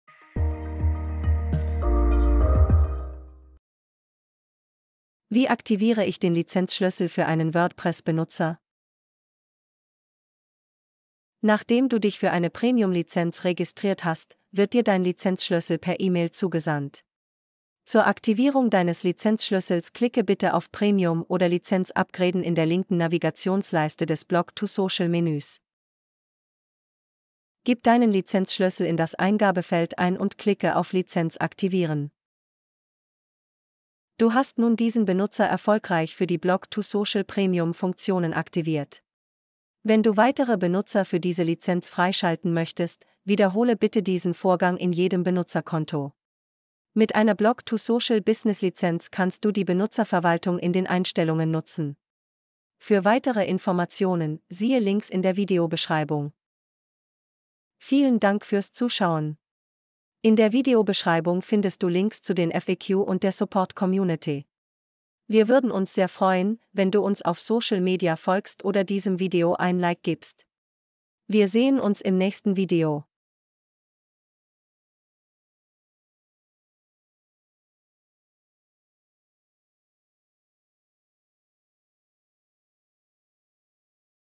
Wie aktiviere ich den Lizenzschlüssel für einen WordPress Benutzer?
Wie kann ich den Lizenzschlüssel bei Blog2Social aktivieren?

Bleib dran für weitere Videos über Social Media Strategien, Management und hilfreiche Anleitungen wie diese!